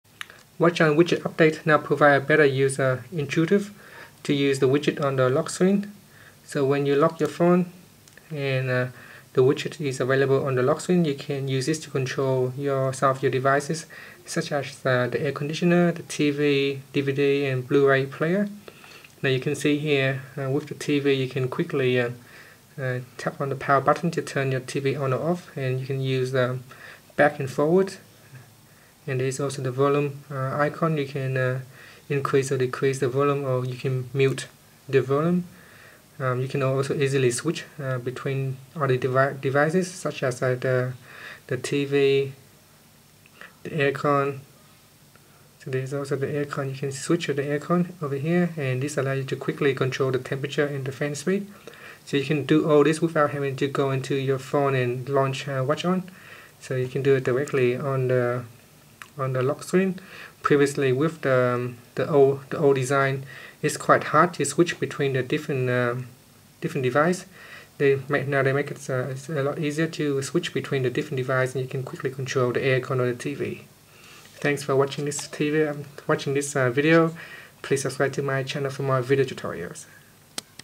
Samsung Galaxy S4: Updated WatchOn Widget on the Lock Screen With Better User Interface
See the new WatchOn Widget that get updated and now it is easier to use on the lock screen of Samsung Galaxy S4.